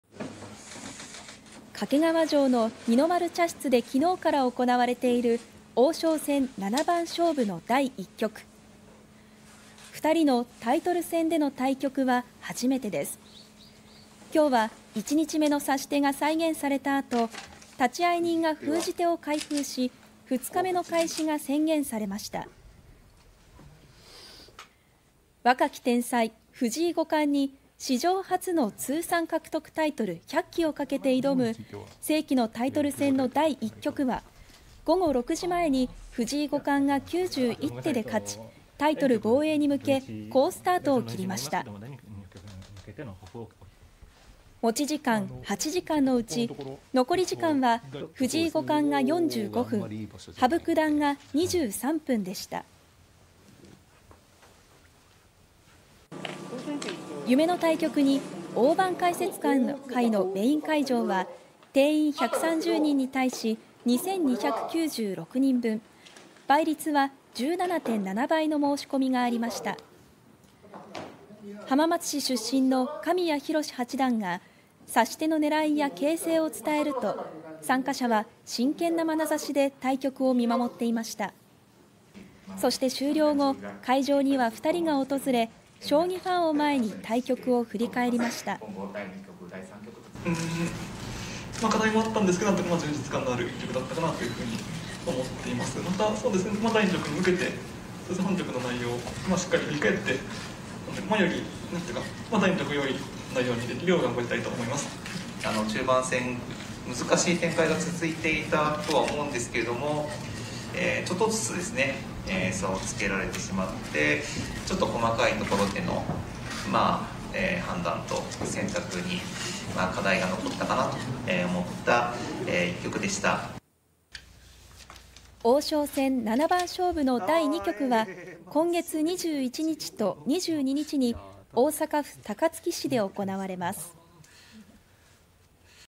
藤井五冠「充実感のある一局」羽生九段「細かい判断と戦略に課題」 王将戦第一局は藤井五冠が勝利
夢の対局で好スタートを切ったのは藤井聡太五冠でした。藤井五冠にレジェンド・羽生善治九段が挑む将棋の王将戦第一局は91手で藤井五冠が勝利しています。

静岡県掛川市の掛川城の二の丸茶室で8日から行われている王将戦七番勝負の第一局。

2人のタイトル戦での対局は初めてです。

9日は1日目の指し手が再現された後 立会人が封じ手を開封し、2日目の開始が宣言されました。

若き天才・藤井五冠に史上初の通算獲得タイトル100期をかけて挑む世紀のタイトル戦の第一局は、午後6時前に藤井五冠が91手で勝ち、タイトル防衛に向け好スタートを切りました。

持ち時間8時間のうち残り時間は藤井五冠が45分、羽生九段が23分でした。

夢の対局に大盤解説会のメイン会場は、定員130人に対し2296人分・倍率17．7倍の申し込みがありました。

浜松市出身の神谷広志八段が、指し手の狙いや形勢を伝えると、参加者は真剣なまなざしで対局を見守っていました。

そして終了後 会場には2人が訪れ、将棋ファンを前に対局を振り返りました。

藤井 聡太 五冠：
課題もあったんですが、充実感のある一局だったかなと思っています。また第二局に向けて本局の内容をしっかり振り返って、前より第一局よりいい内容にできるよう頑張りたいと思います

羽生 善治 九段：
中盤戦、難しい展開が続いていたと思うんですけれども、ちょっとずつ差をつけられてしまって、ちょっと細かいところでの判断と戦略に課題が残ったかなと思った一局でした

王将戦七番勝負の第二局は、1月21日と22日に大阪府高槻市で行われます。